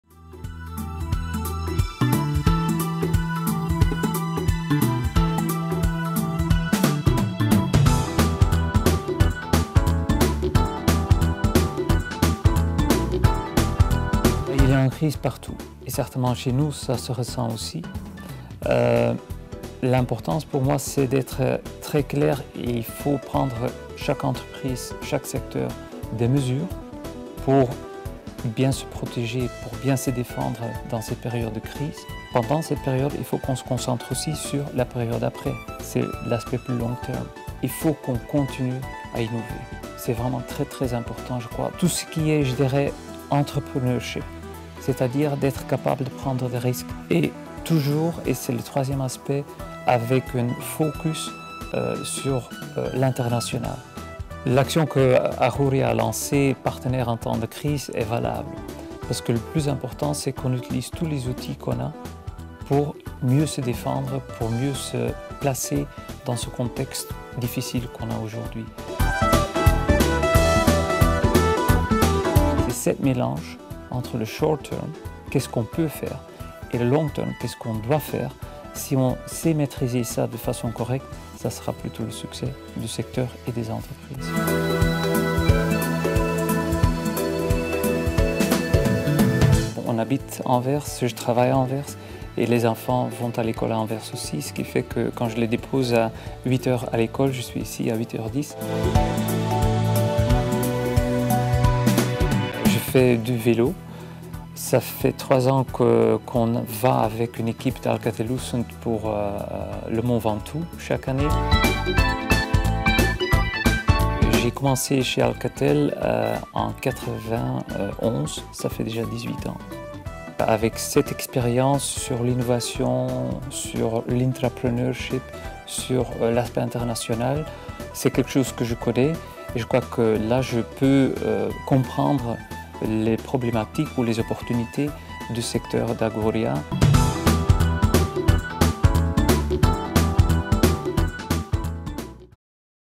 Paul Depuydt, nouveau président d'Agoria
portrait de Paul Depuydt, CEO d'Alcatel-Lucent Belgique et nouveau président d'Agoria, al fédération de l'industrie technologique